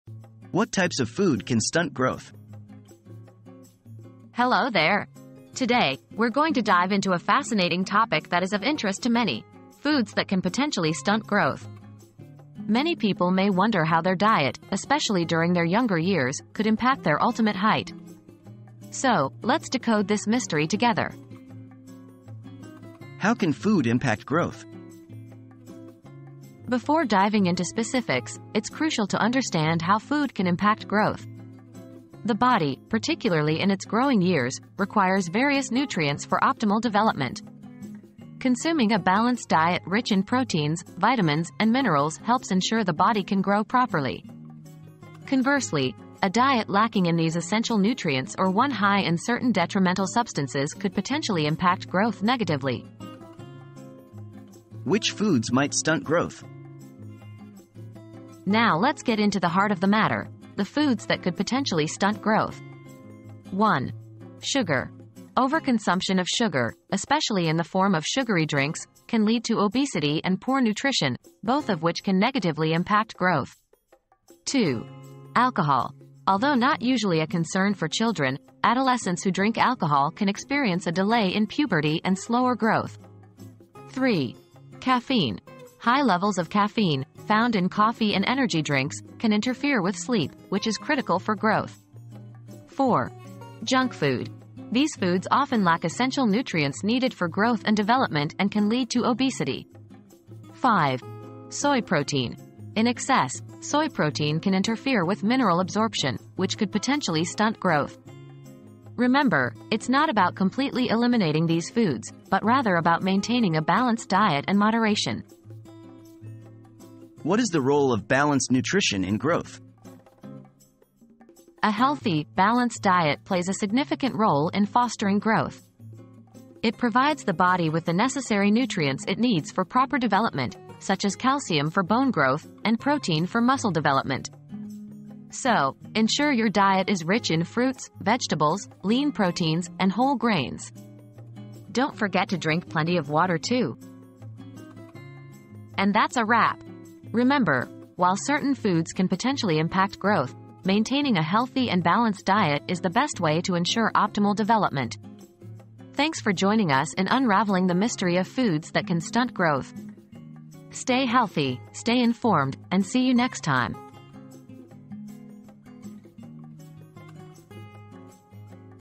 What types of food can stunt growth?
Unraveling the Mystery: Foods That Can Stunt Growth • Are you curious about what foods could potentially stunt your growth? Look no further! In this video, we uncover the mystery behind the types of food that could impact your height. From sugar to soy protein, we explore which foods to consume in moderation for optimal growth. Join us as we dive into the fascinating world of nutrition and growth!

00:00 • What types of food can stunt growth?
00:23 • How Can Food Impact Growth?
00:54 • Which Foods Might Stunt Growth?
01:57 • What is the Role of Balanced Nutrition in Growth?